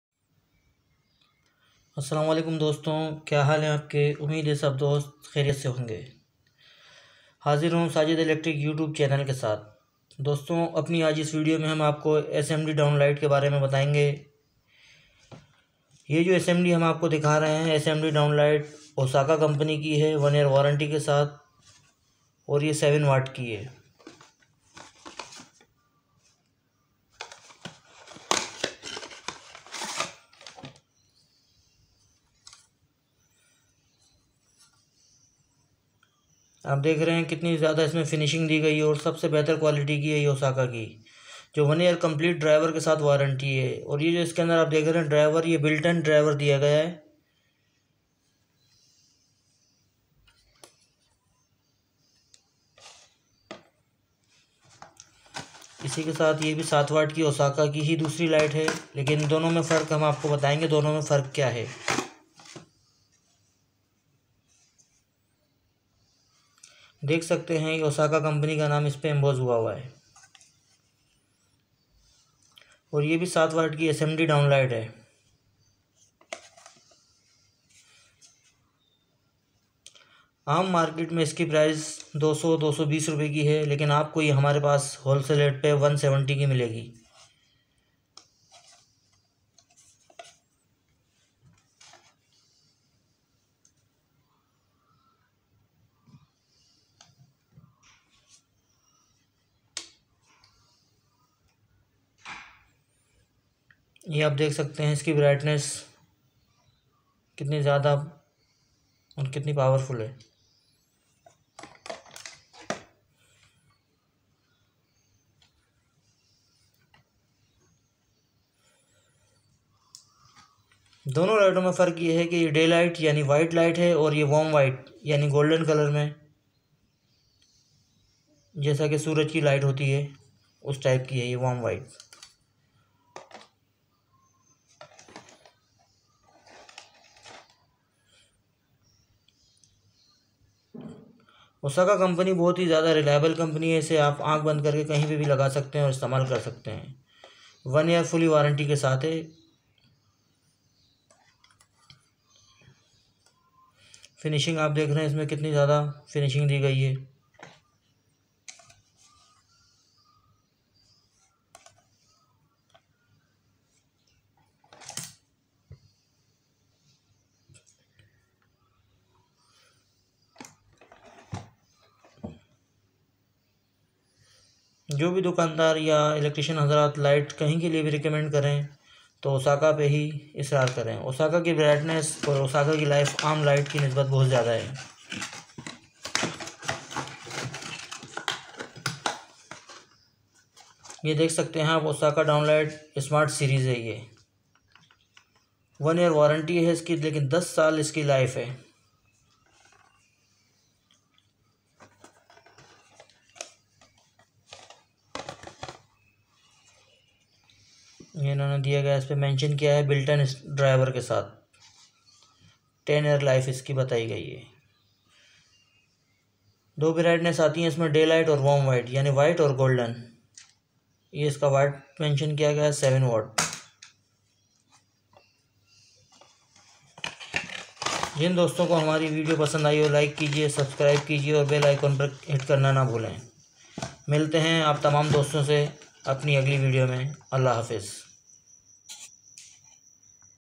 LED CONCEILD DOWNLIGHTS 7 WATTS /D,L/W,W
Best Led down light in Pakistan 2021 | muliti colour /daylight/warm white

Hello, friends, My name is Amir Hussain with lots of Experience in this Electric field.
Today I'll show Led Downlight in Pakistan 2021 | downlights forconceild/open .

All videos here on Sajid Electric Channel.